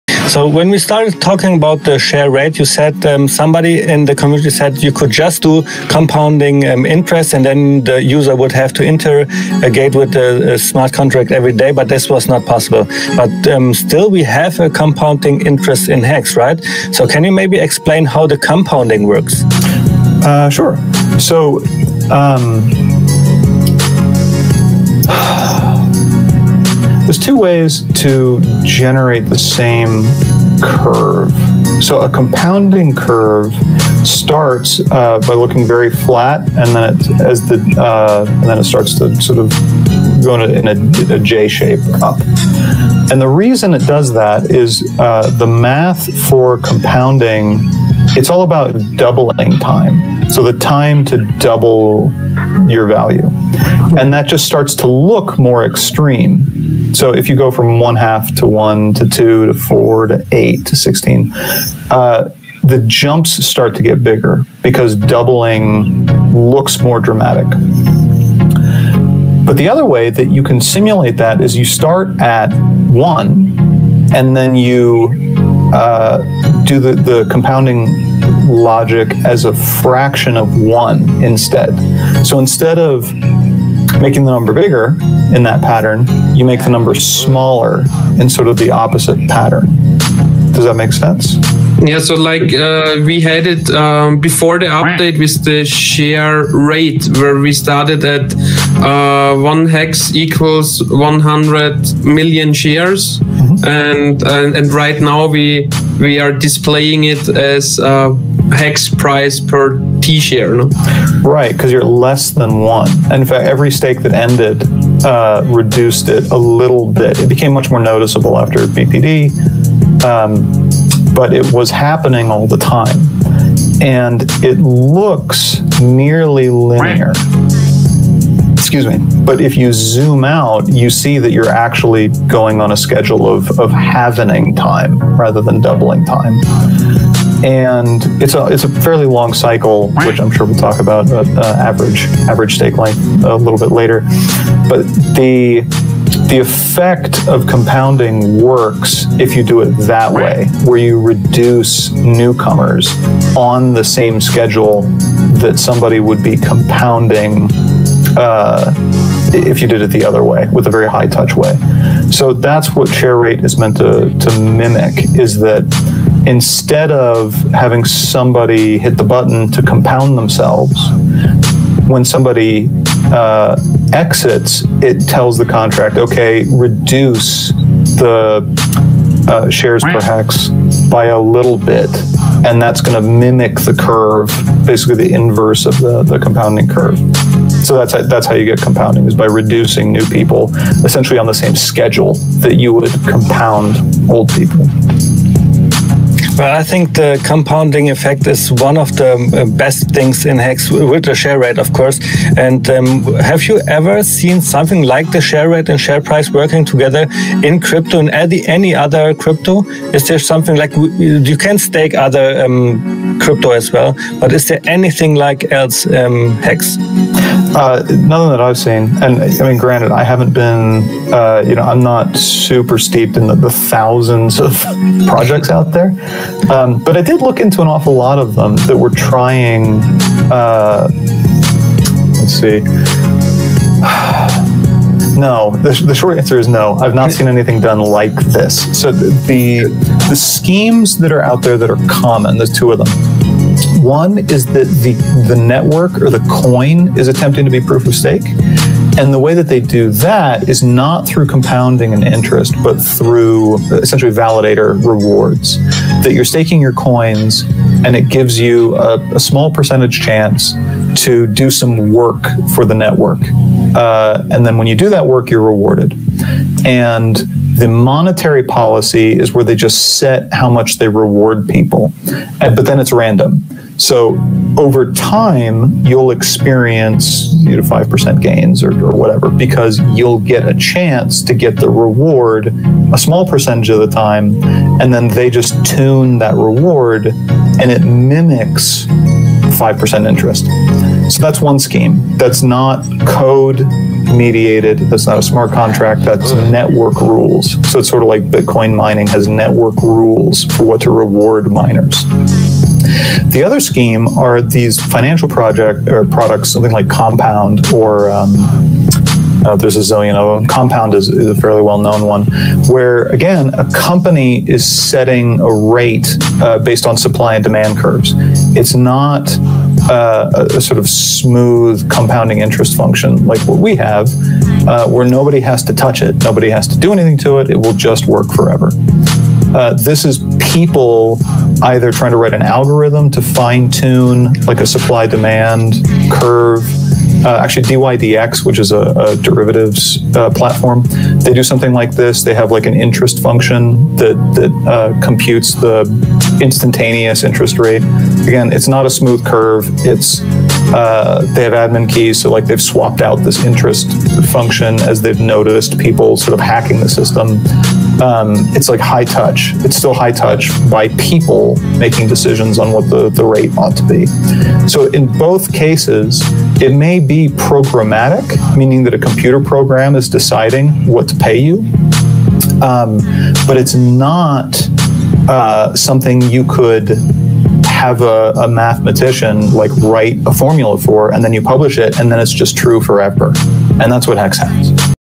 Dev Kyle Talks About How HEX Shares (How Shares/Share Rate Compounds) & Share Price Work
Dlive.tv/BallietBran

Youtube.com/BallietBran

Pscp.tv/SixInBrian

Twitch.tv/BallietBran

Twitter.com/SixInBrian

Twitter.com/BallietBran

(David Feder HEX Success Story)

Hex.com